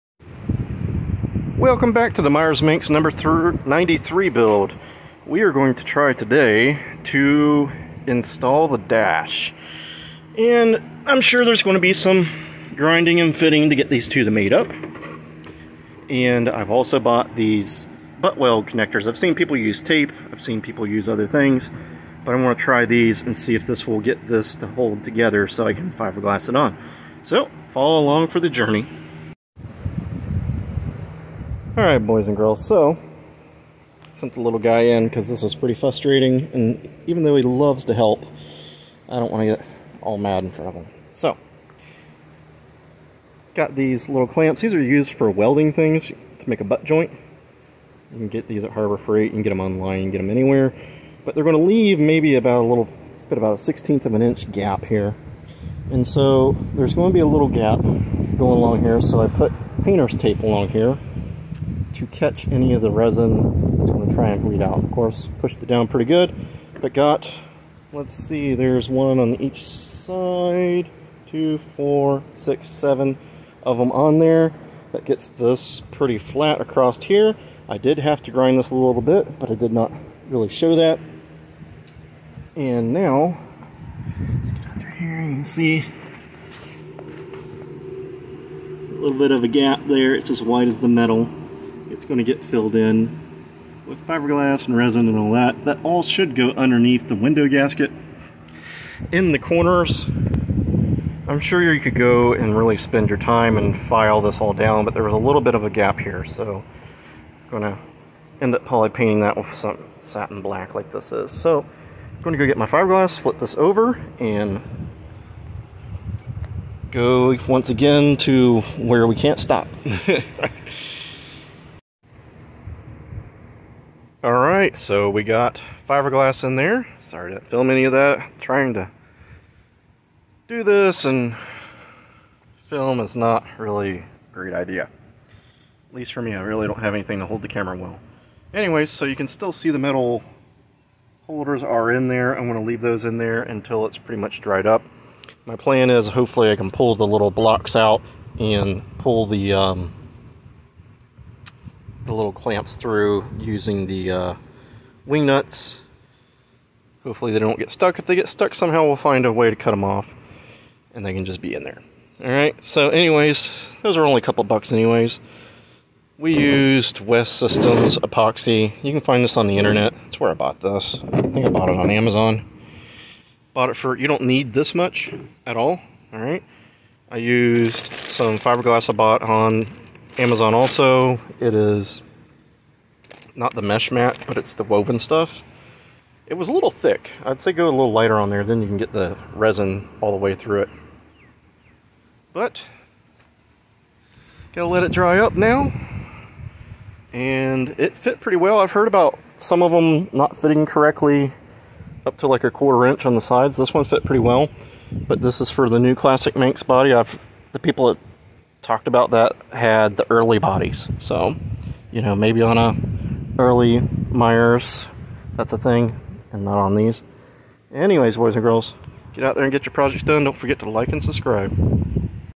Attaching the Manxs dash to the hood.
Fiberglassed the dash to the hood today. I've only seen one or two people do this and learned from their short comings, not that I did a stellar job at it either LOL. Hopefully it will gives others an idea on what to do. If I had to do it again I would tape all the seams together and poke the but connector clamps through the tape and grease the clamps so they will release easily.